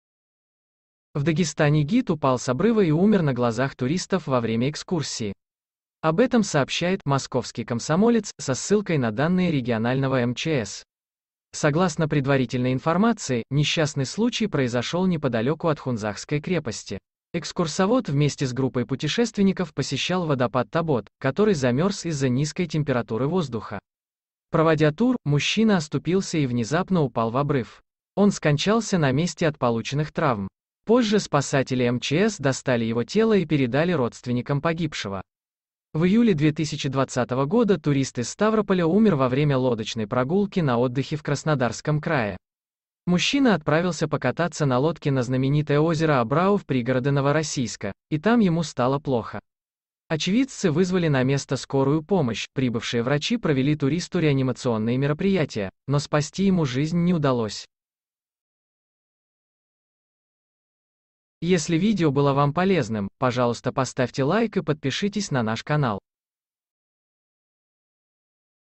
В Дагестане гид умер на глазах туристов во время экскурсии
В Дагестане гид упал с обрыва и умер на глазах туристов во время экскурсии. Согласно предварительной информации, несчастный случай произошел неподалеку от Хунзахской крепости. Экскурсовод вместе с группой путешественников посещал водопад Тобот, который замерз из-за низкой температуры воздуха.